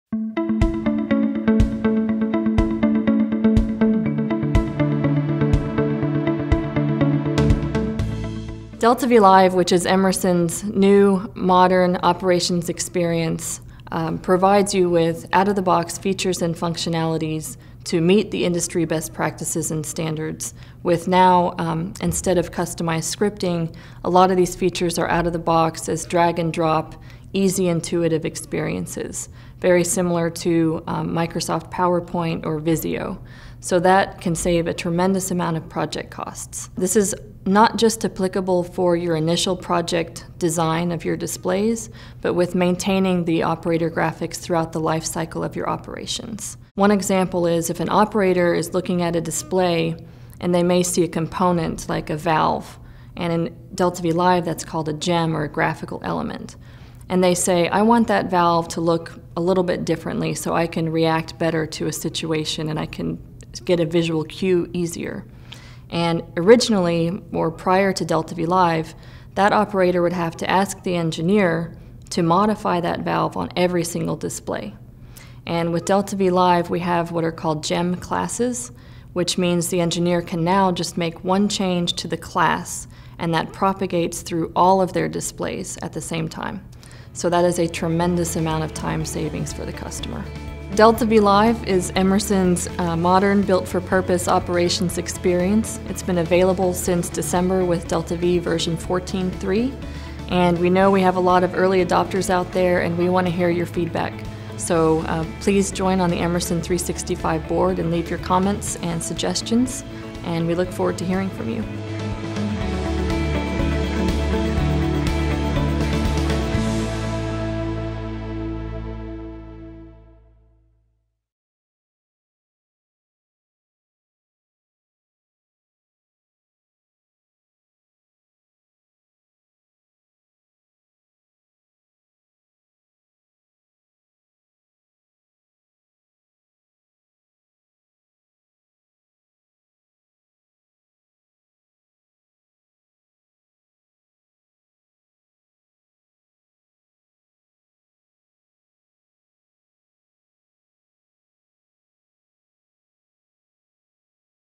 Reduce Project Costs with DeltaV™ Live HMI Configuration and Optimization
Emerson’s modern DeltaV HMI, is built with out-of-the-box features that reduce engineering configuration effort & project costs - providing innovative technology for enhanced flexibility.